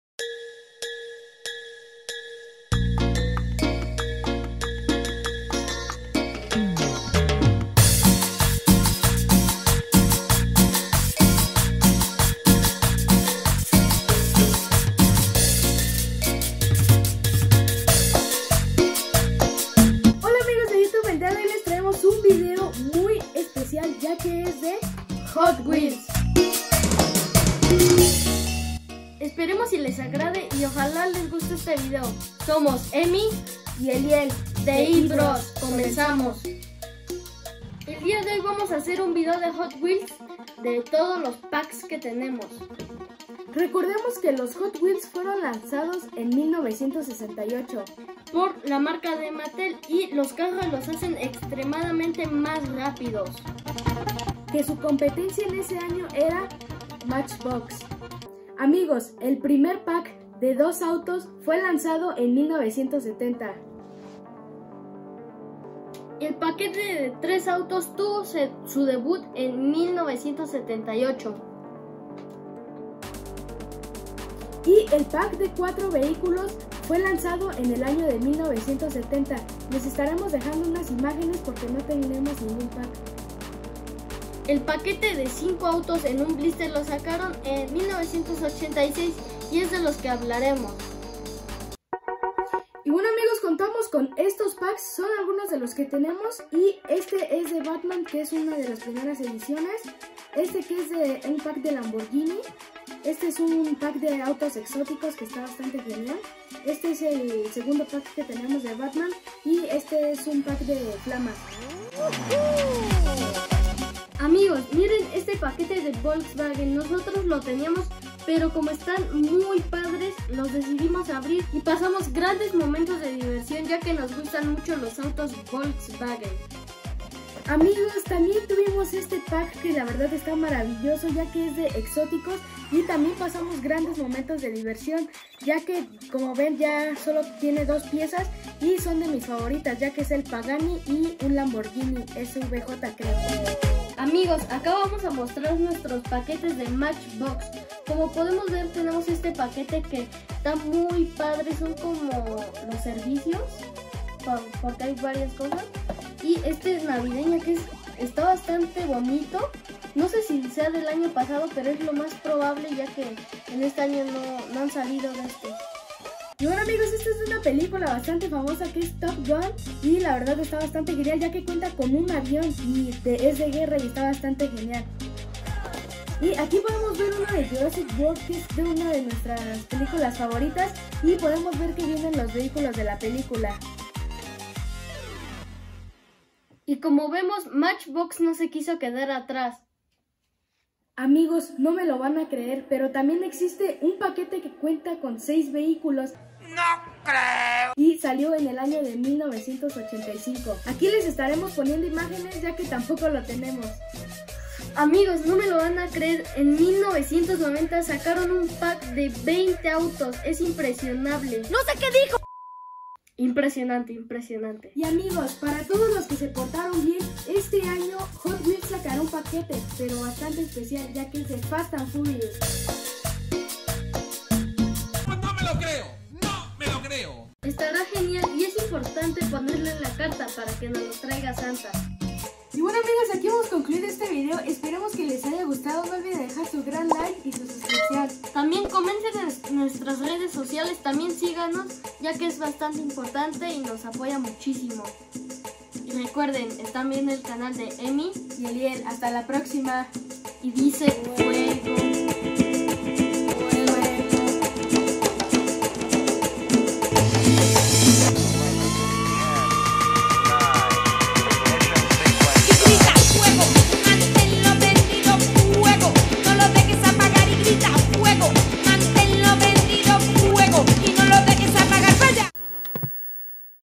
Hot wheels 5 pack DATOS QUE NO CONOCÍAS🚘🛺🚙
En este video dejamos los años y algunas curiosidades sobre los packs de HotWheels y Matchbox esperamos que sea de tu agrado y aumentes tu Coleccion.
Gracias por apoyarnos y dejar tu like

┗┫┏━━┓┣┛╲╰┳╮!! ¡ES GRATIS!!-┃